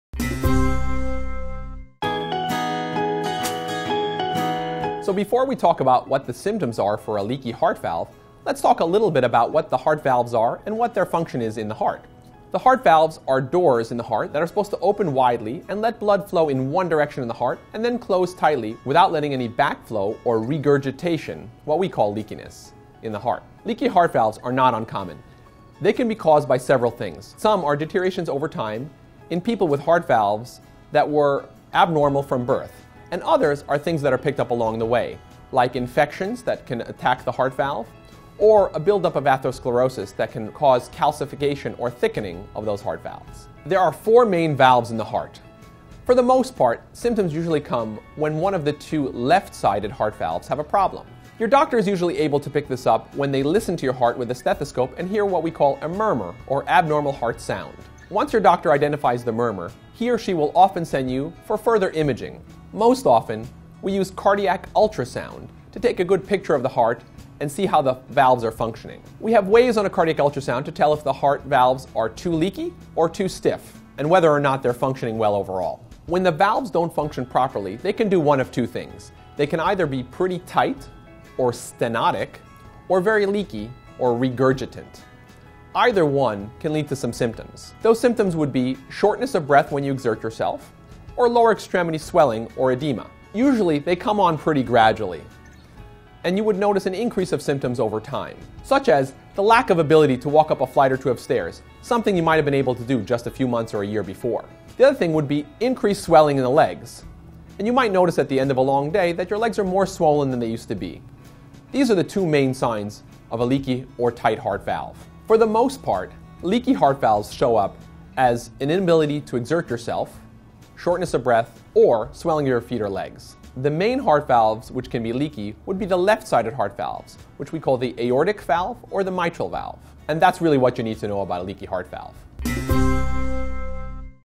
2 Symptoms of a Leaking Heart Valve | Heart Disease
So, before we talk about what the symptoms are for a leaky heart valve, let's talk a little bit about what the heart valves are, and what their function is in the heart. The heart valves are doors in the heart that are supposed to open widely and let blood flow in one direction of the heart, and then close tightly, without letting any back flow, or regurgitation, what we call leakiness in the heart.

Leaky heart valves are not uncommon. They can be caused by several things. Some are deteriorations over time, and people with heart valves that were abnormal from birth, and others are things that are picked up along the way, like infections that can attack the heart valve, or a buildup of atherosclerosis that can cause calcification, or thickening of those heart valves.

There are four main valves in the heart. For the most part, symptoms usually come when one of the two left-sided heart valves have a problem. Your doctor is usually able to pick this up when they listen to your heart with a stethoscope and hear what we call a murmur, or abnormal heart sound. Once your doctor identifies the murmur, he or she will send you for further imaging. Most often, we use cardiac ultrasound to take a good picture of the heart and see how the valves are functioning. We have waves on the cardiac ultrasound to tell us the heart valves are too leaky or too stiff and whether or not they're functioning well overall.

When valves don't function properly, they can do one of two things. They can either be pretty tight, or stenotic, or leaky, or regurgitant. Either one can lead to some symptoms. Those symptoms would be shortness of breath when you exert yourself, or lower extremity swelling, or edema. Usually, they come on pretty gradually and you would notice an increase of symptoms over time such as the lack of  ability to walk up a flight or two of stairs, something you might have been able to do just a few months or a year before. The other thing would be increased swelling in the legs. And you might notice at the end of a long day that your legs are more swollen than they used to be. These are the two main signs of a leaky or tight heart valve.

 For the most part, leaky heart valves show up as an inability to exert yourself, shortness of breath, or swelling of your feet or legs. The main heart valves which can be leaky would be the left-sided heart valves, which we call the aortic valve or the mitral valve. And that's what you really need to know about a leaky heart valve.